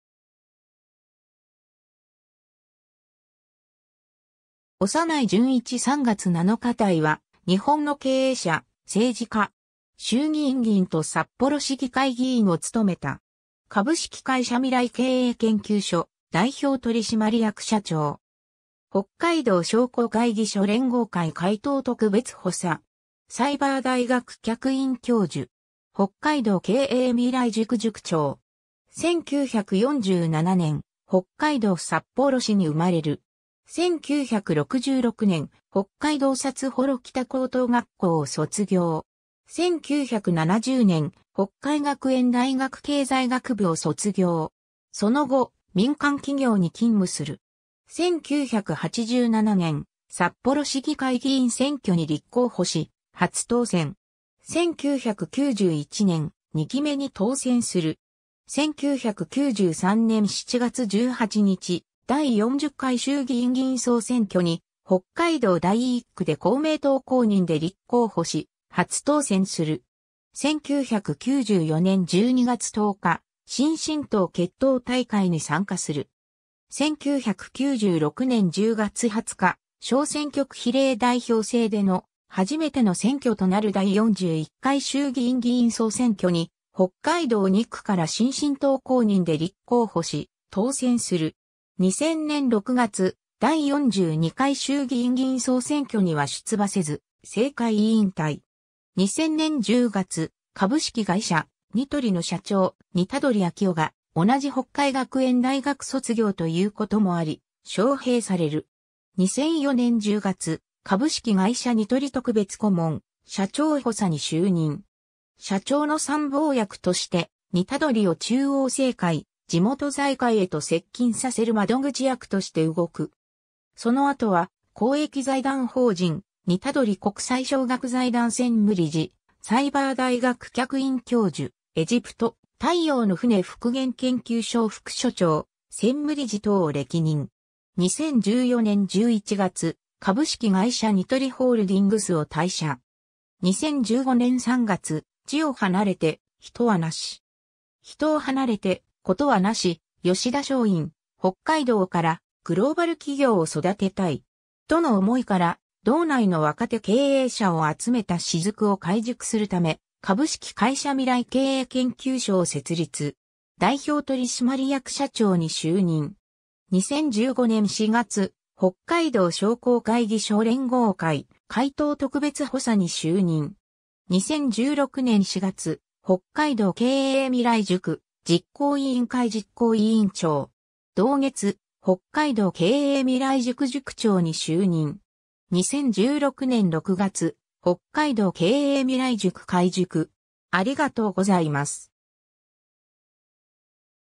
長内順一
長内 順一（おさない じゅんいち、1947年（昭和22年）3月7日 - ）は、日本の経営者、政治家。
衆議院議員（2期）と札幌市議会議員（2期）を務めた。
 株式会社未来経営研究所代表取締役社長。
北海道商工会議所連合会会頭特別補佐。
サイバー大学客員教授（企業社会責任論）。
北海道経営未来塾塾長。
 1947年（昭和22年）、北海道札幌市に生まれる。
1966年（昭和41年）、北海道札幌北高等学校を卒業。
1970年（昭和45年）、北海学園大学経済学部を卒業。
その後、民間企業に勤務する。
 1987年（昭和62年）、札幌市議会議員選挙に立候補し、初当選。
1991年（平成3年）、2期目に当選する。
 1993年（平成5年）7月18日、第40回衆議院議員総選挙に、北海道第1区で公明党公認で立候補し、初当選（以降、連続2期）する。
1994年（平成6年）12月10日、新進党結党大会に参加する。
1996年（平成8年）10月20日、小選挙区比例代表制での初めての選挙となる第41回衆議院議員総選挙に北海道2区から新進党公認で立候補し当選する。
 2000年（平成12年）6月、第42回衆議院議員総選挙には出馬せず政界引退。
 2000年10月、株式会社ニトリの社長・似鳥昭雄が同じ北海学園大学卒業と言うこともあり、招聘される。
2004年（平成16年）10月、株式会社ニトリ特別顧問・社長補佐に就任。
「社長の参謀役」として、似鳥を中央政界・地元財界へと接近させる窓口役として動く。
その後は公益財団法人・似鳥国際奨学財団専務理事、サイバー大学客員教授（企業社会責任論）、エジプト「太陽の船復元研究所」副所長・専務理事等を歴任。
2014年（平成26年）11月、株式会社ニトリホールディングスを退社。
 2015年（平成27年）3月、～地を離れて人はなし。
人を離れて事はなし！：吉田松陰～「北海道からグローバル企業を育てたい。」との思いから、道内の若手経営者を集めた私塾を開塾するため、株式会社未来経営研究所を設立。
代表取締役社長に就任。
2015年（平成27年）4月、北海道商工会議所連合会会頭特別補佐に就任。
 2016年（平成28年）4月、北海道経営未来塾実行委員会（北海道商工会議所連合会、札幌商工会議所、北洋銀行、北海道銀行、未来経営研究所　後援：札幌市）実行委員長。
同月、北海道経営未来塾　塾長に就任。
2016年6月、「北海道経営未来塾」開塾。